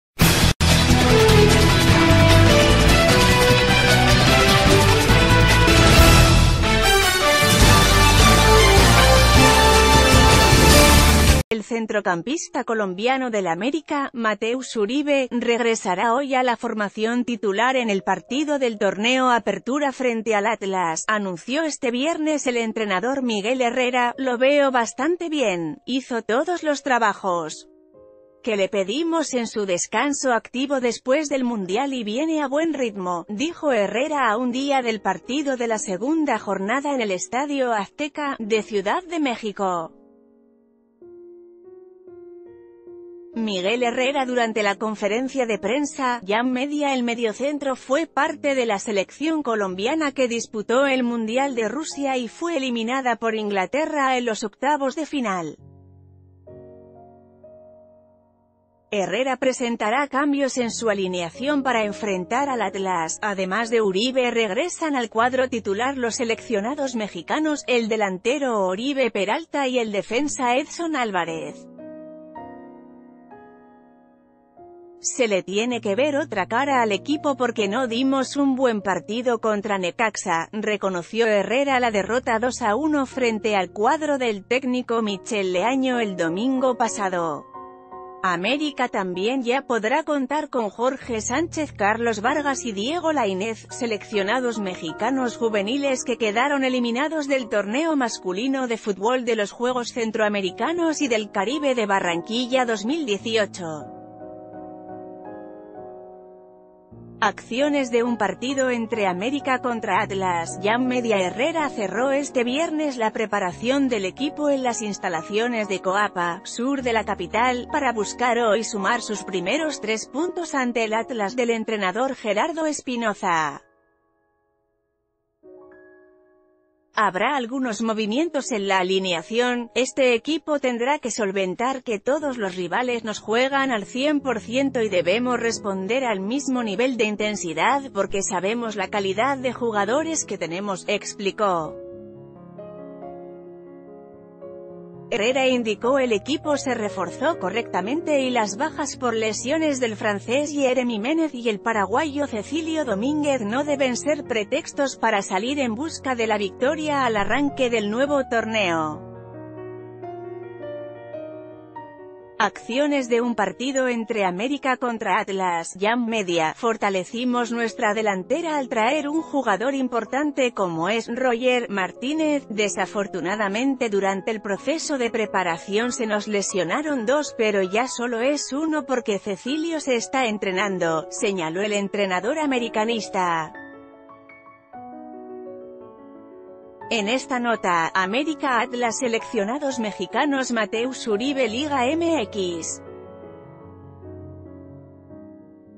América enfrentará a Atlas con seleccionados y Mateus Uribe
El centrocampista colombiano del América, Mateus Uribe, regresará hoy a la formación titular en el partido del torneo Apertura frente al Atlas, anunció este viernes el entrenador Miguel Herrera.“Lo veo bastante bien, hizo todos los trabajos que le pedimos en su descanso activo después del Mundial y ...